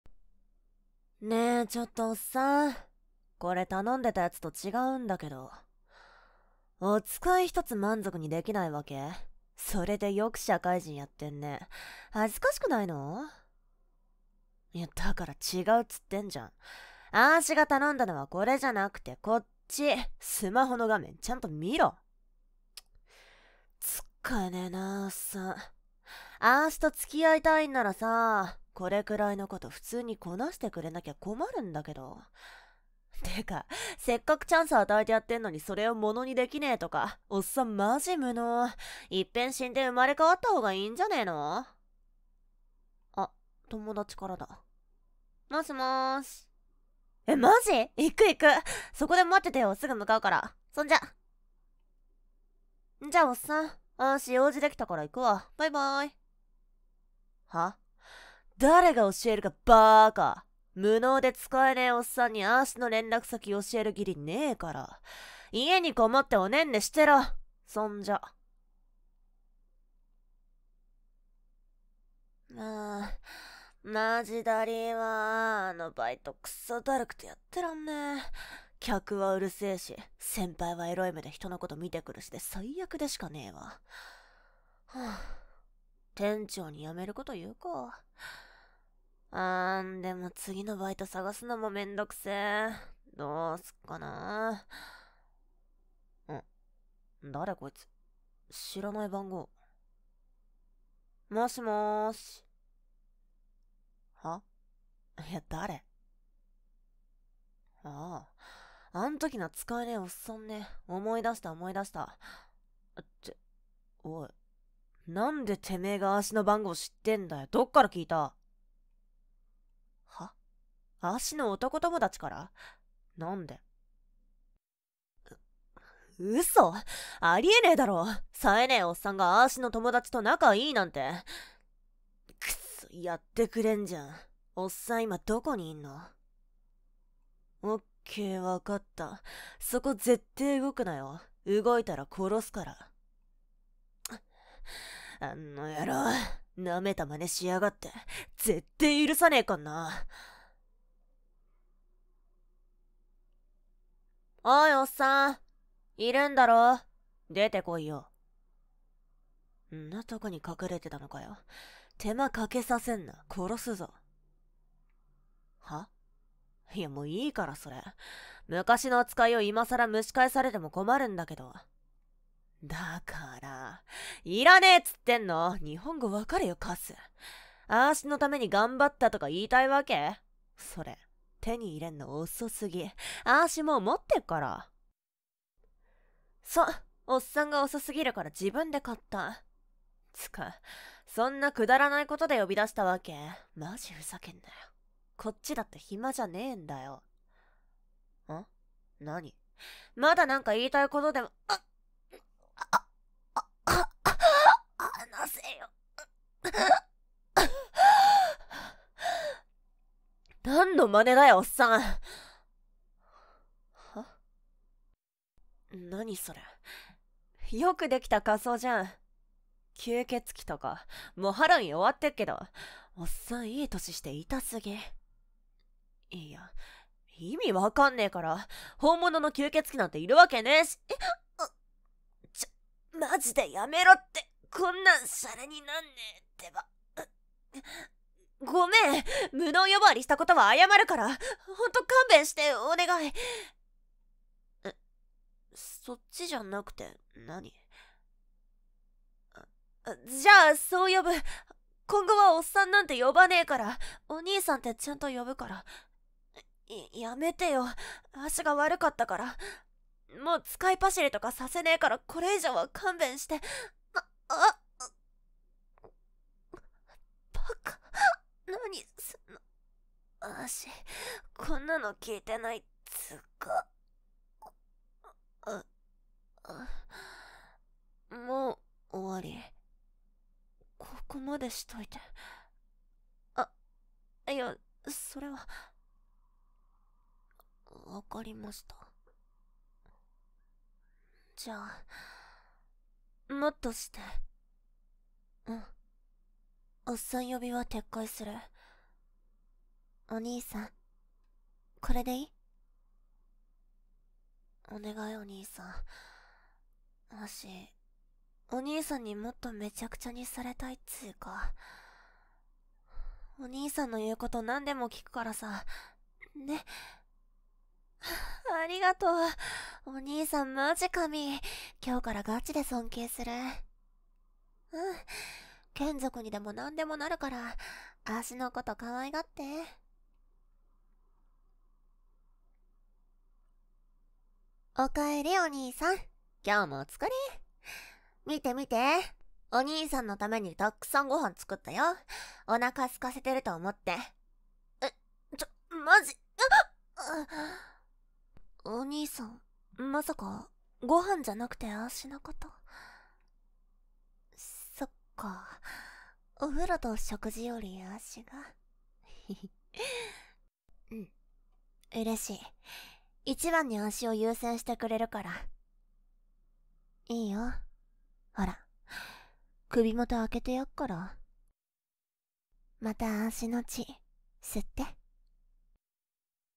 【わからせ】クソ生意気なギャルの女の血を吸血してわからせたら眷属奴隷になった【男性向けシチュエーションボイス】cv:安藤ナツ
▼あらすじ
吸血鬼の貴方は眷属にしたい女を見つけ、しばらくは彼女の言うことを何でも聞いていた。しかし、そろそろ我慢の限界に達した貴方は彼女を人気のないところに呼び出し、自らの正体を晒して襲い掛かるのだった…

つかえねー(笑)

▼使用させていただいた台本
松平蒼太郎様
素敵な台本をありがとうございます！

▼イラスト
あれっくす様
素敵なイラストをありがとうございます！